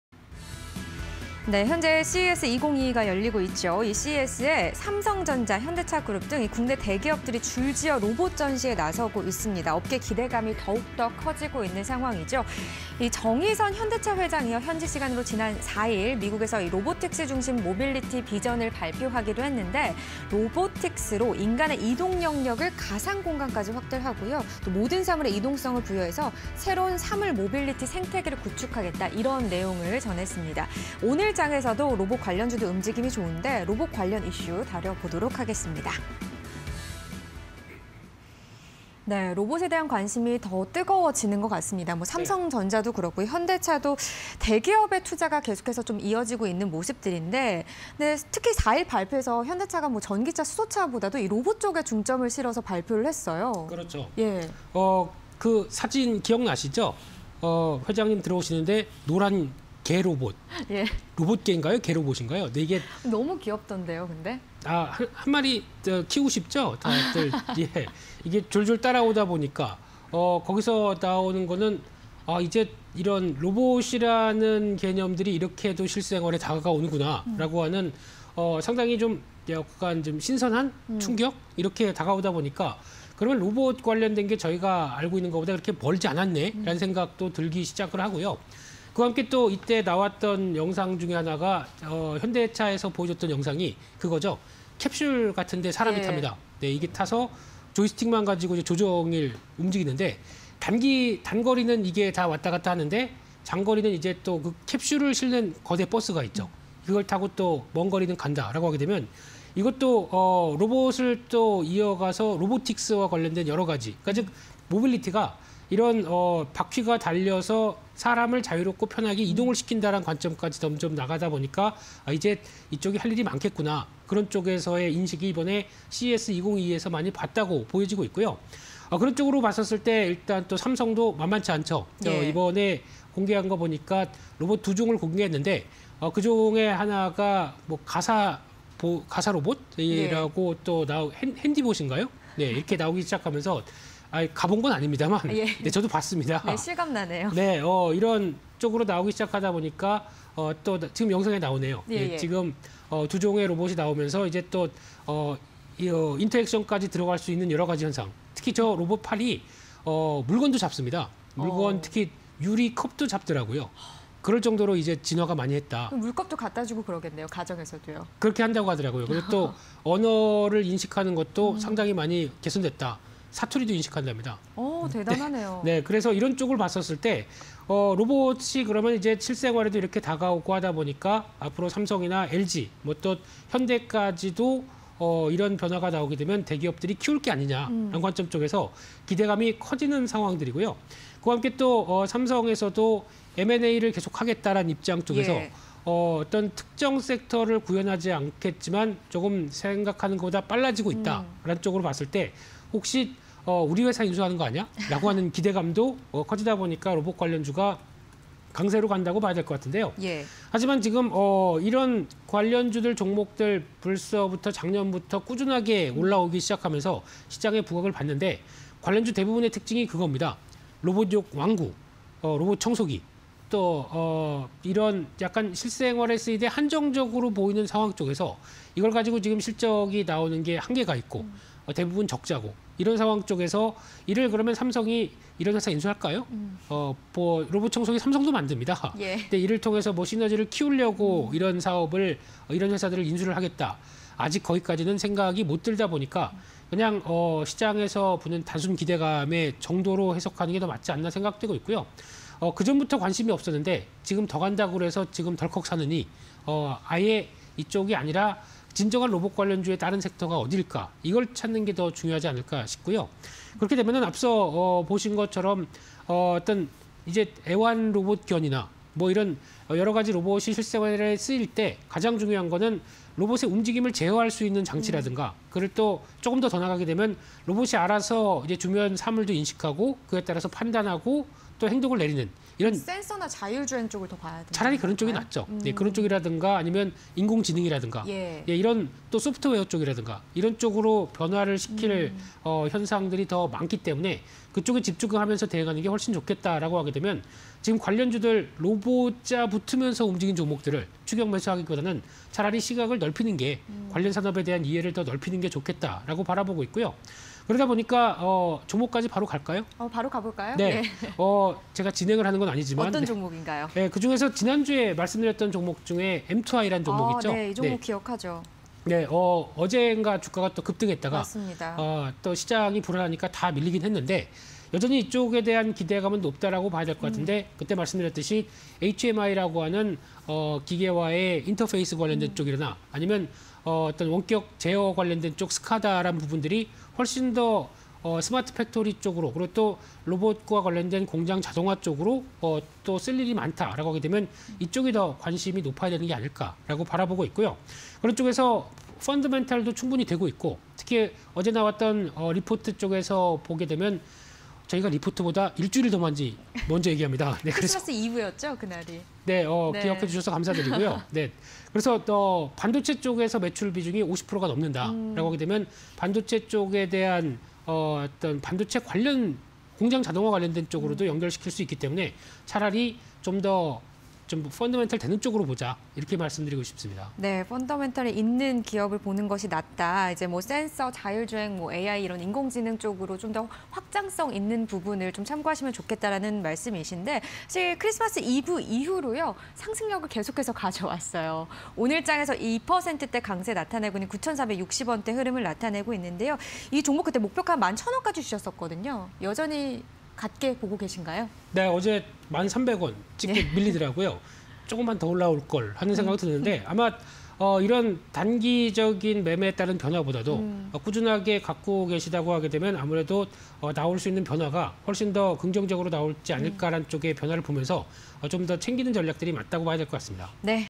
엠투아이(347890)... 삼성·현대·LG 등 'CES 2022'에서 로봇 전시_이슈 팩토리 (20220107)
# 대기업 투자 확대되나… 훈풍 부는 '로봇주'
- 삼성·현대·LG 등 로봇사업 확대 기대감↑
- 'CES 2022'에서 신로봇 출시
- '뉴삼성' M&A 기대감 확대
- AI 및 스마트팩토리 시장 확대… 실질적 수혜 가능

엠투아이(347890)... 반도체 투자 확대 따른 수혜 기대
- 스마트팩토리의 기계조작 대시보드 개발·제조
- 다양한 산업군 스마트팩토리 시설 납품
- 삼성전자·SK하이닉스 등 주요 고객사 보유
- 실적 확장세 지속… 지난해 매출 395억(+18.9%)
- 반도체, 전체 매출 55% 차지